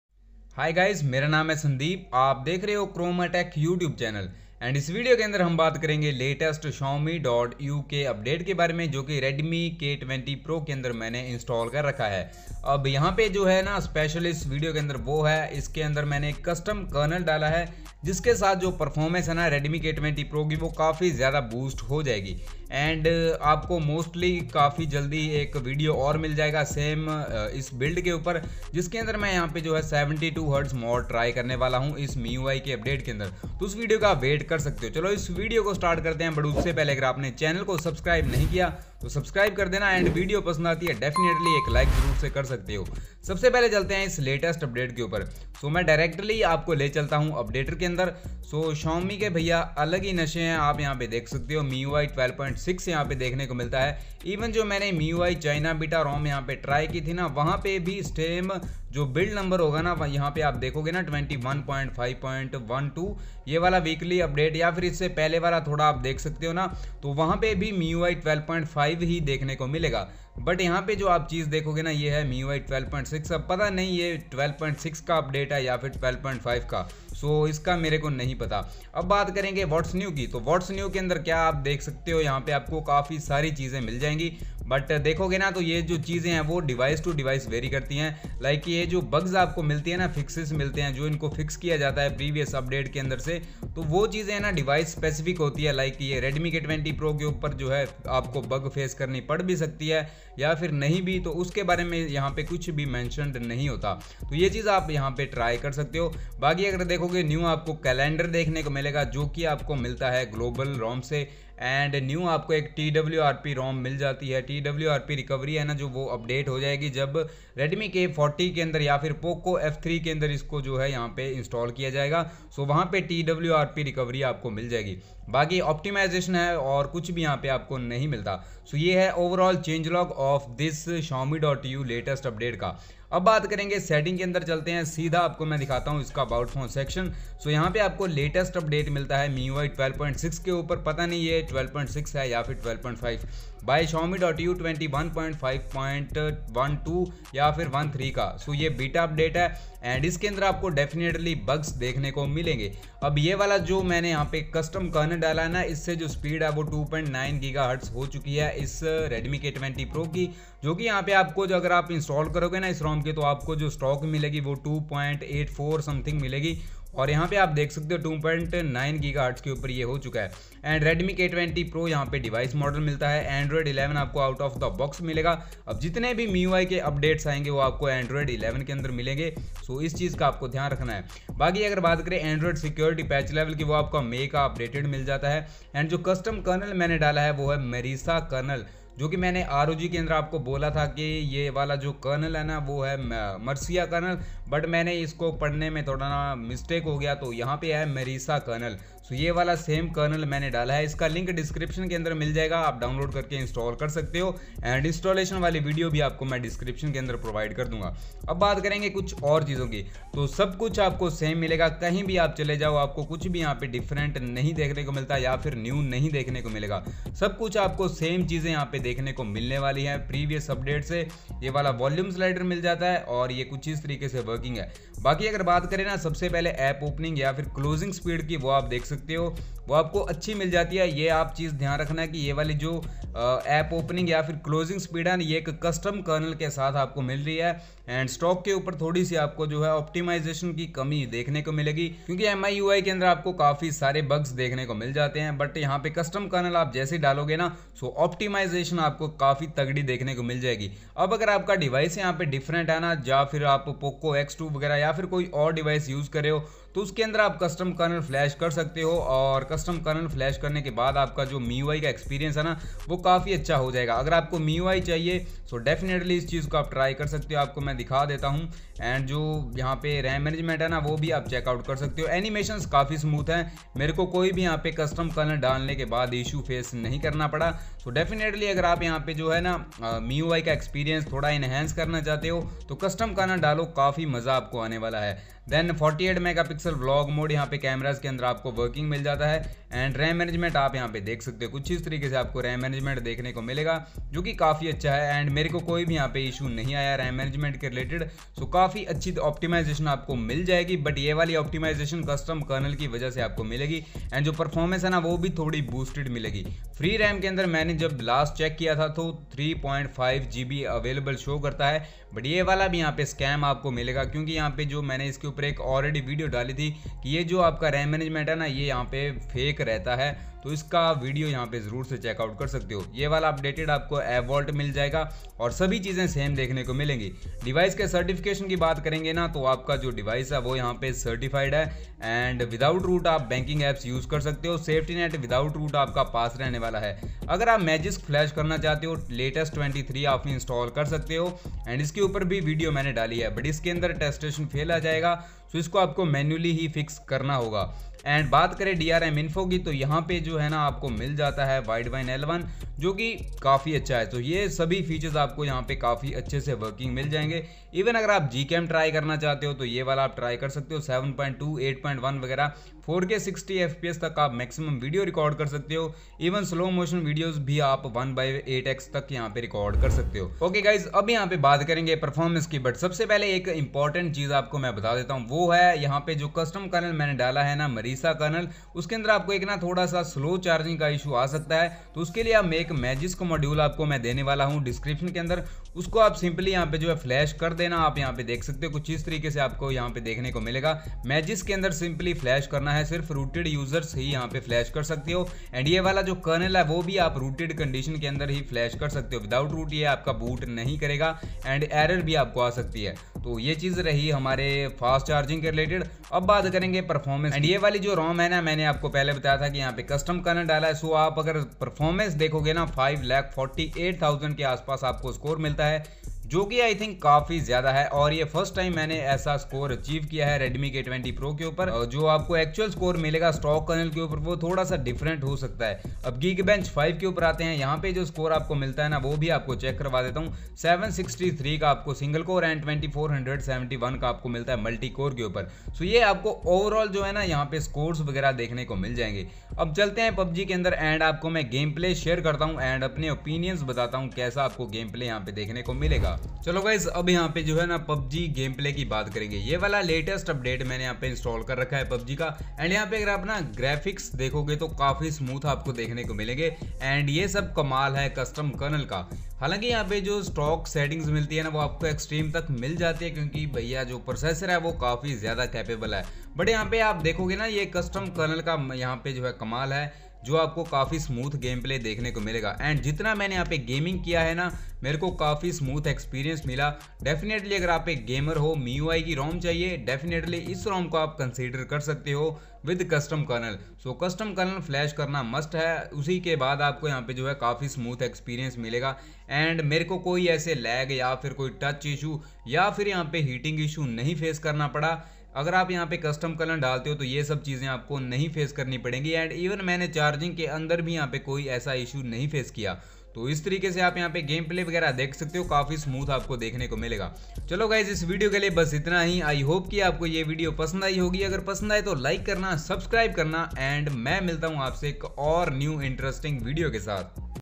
MIUI 12.6 ANDROID 11 Update For Redmi K20 Pro by Xiaomi EU 21.5.12/13 With Custom Kernel 🔥
♪Beat made by Chunki Beats
                  S U B S C R I B E & L I K E